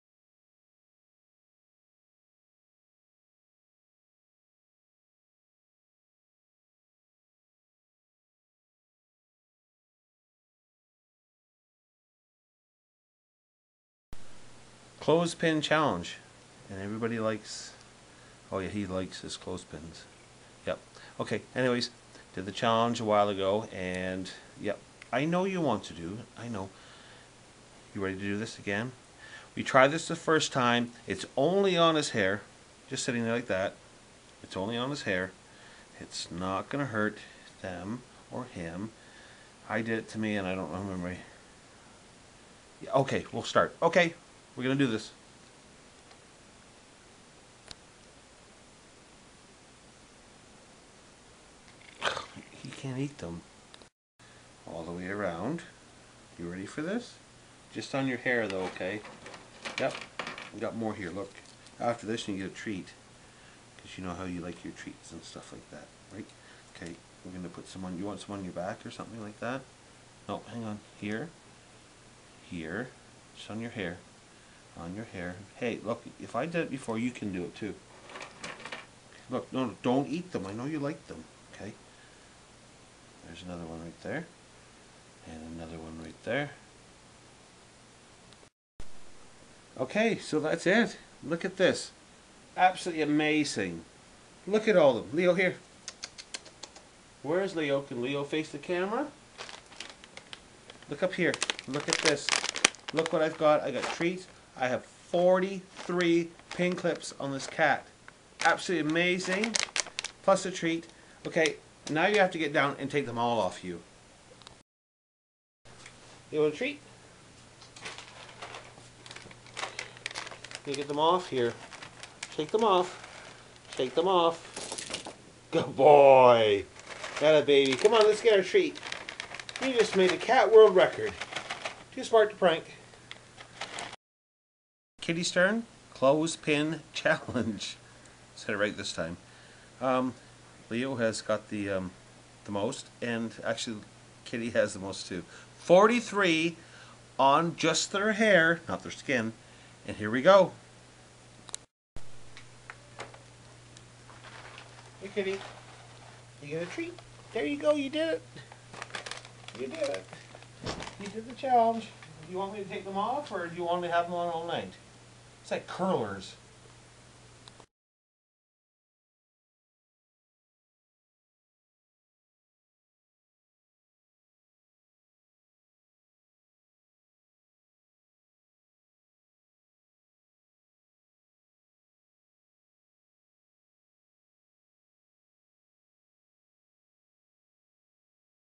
Cat Clothespin Challenge
My husband did the Challenge and we thought we would try it on Leo and Kitty. Our cats are not harmed in this challenge, the clothespins are placed on their fur only.